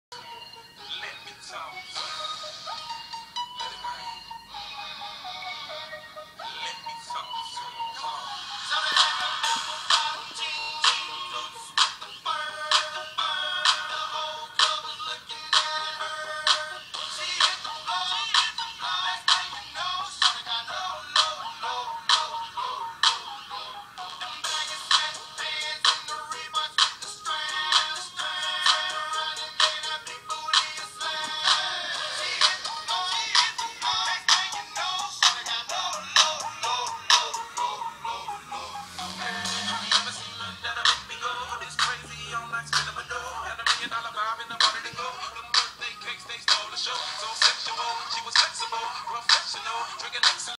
rc trailer part 3
i have now added 8 LEDs and they also flash to the music